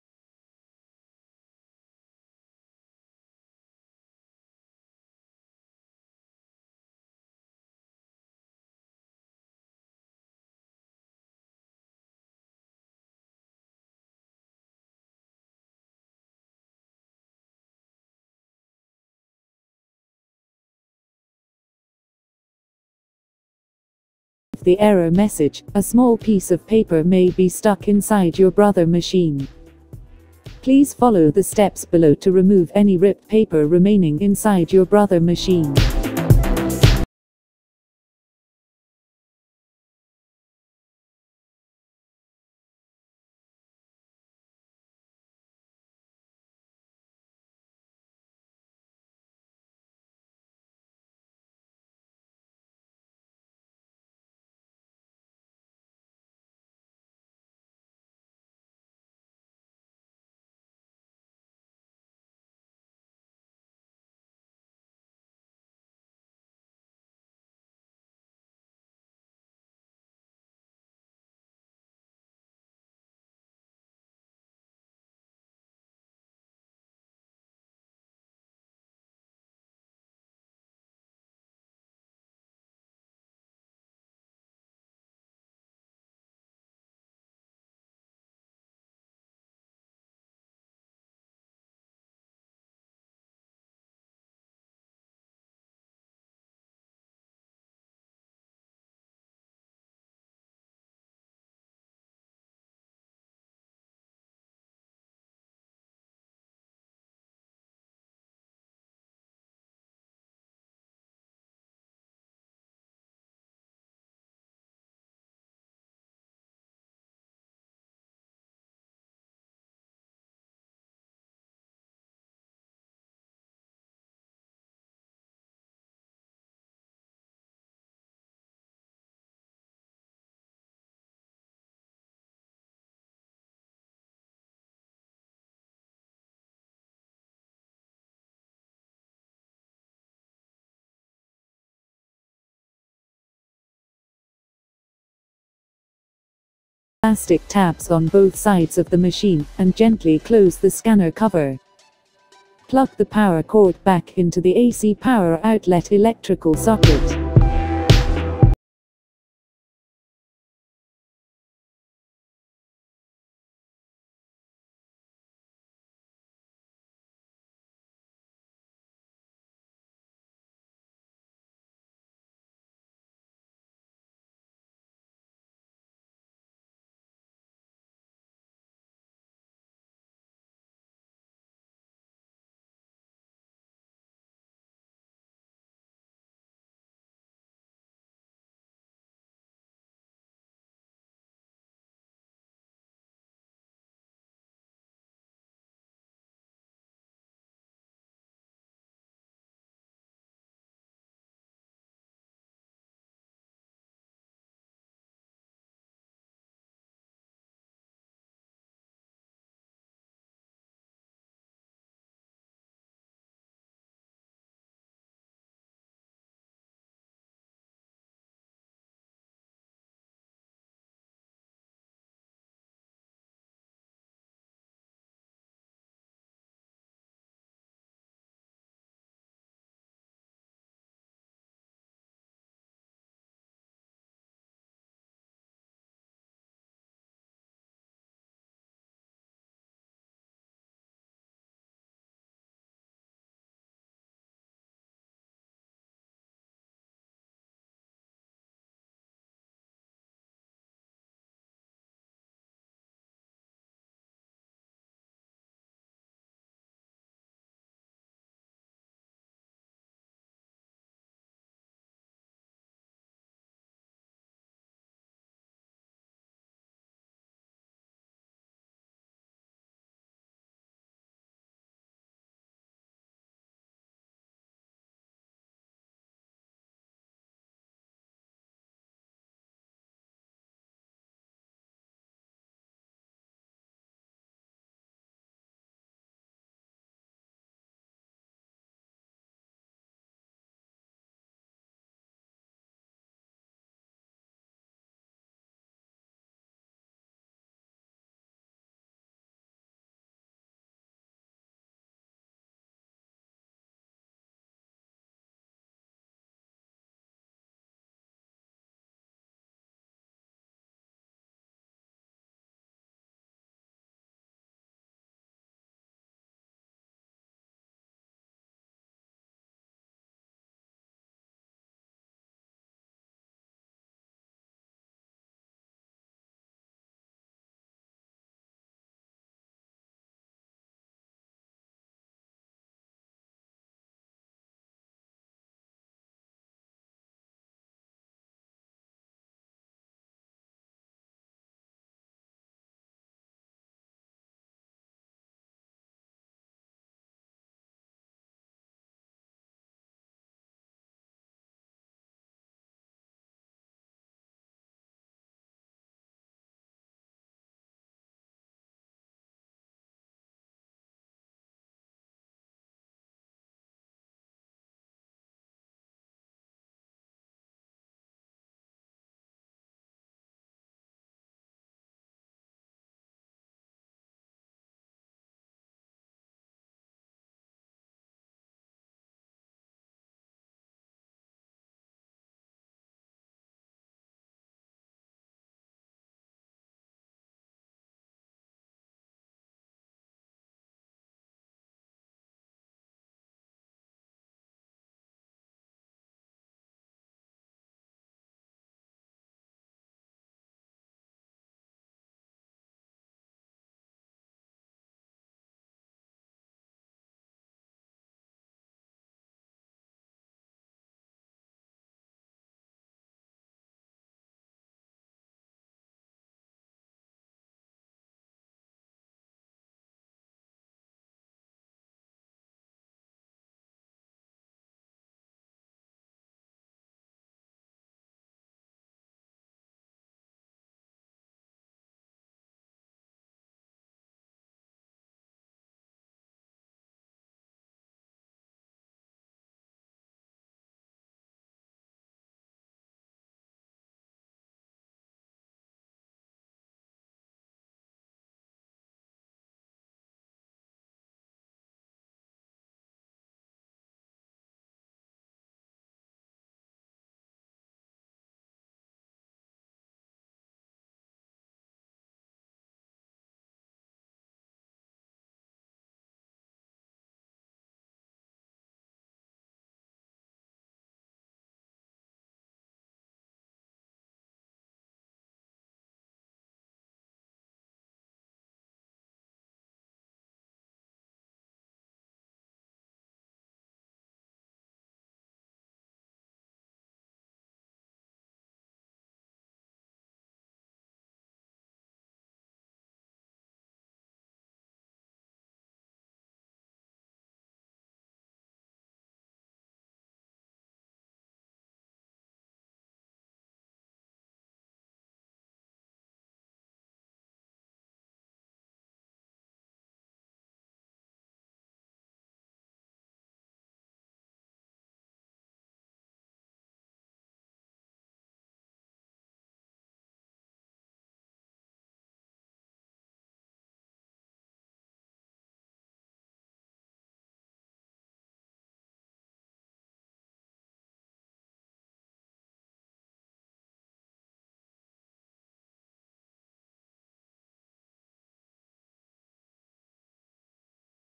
Brother mfc-j825dw Fixing Paper Jam
Brother mfc-j825dw Fixing Paper Jam print Error Solution, Brother MFC-J825 Paper Jams  solution error this here tutorial paper error